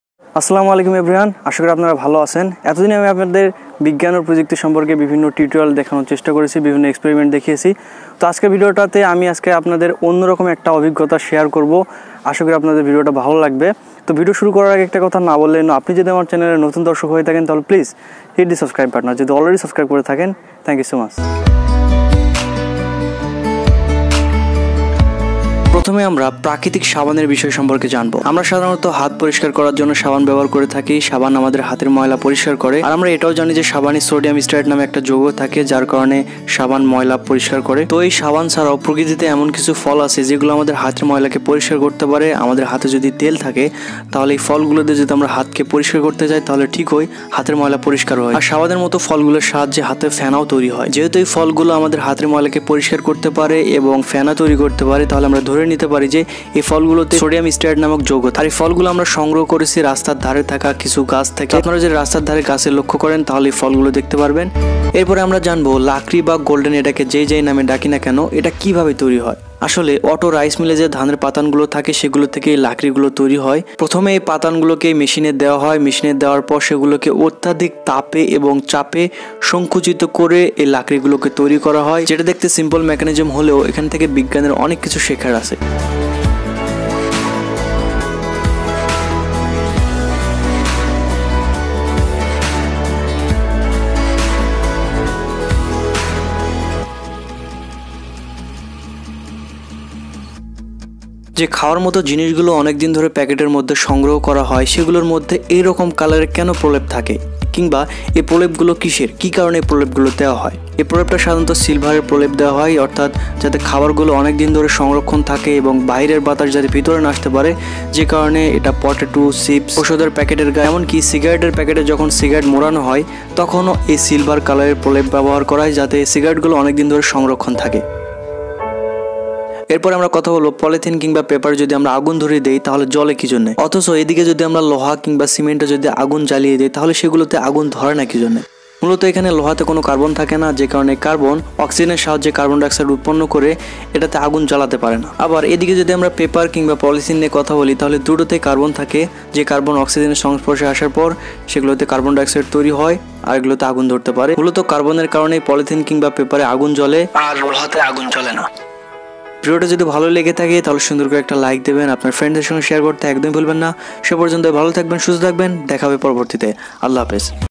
চারপাশের অবহেলিত বিজ্ঞান | The neglected Science Around You!
Basically, this is a educational video.And I know that this such type of educational video people don't like so much.

But I hope you all enjoy this video and gather knowledge.

You will find answer :
Alternative Soap.
How to make Golden/lakri
Why silled chips with Silver layer
Why not Metal/Fe do not burn or fire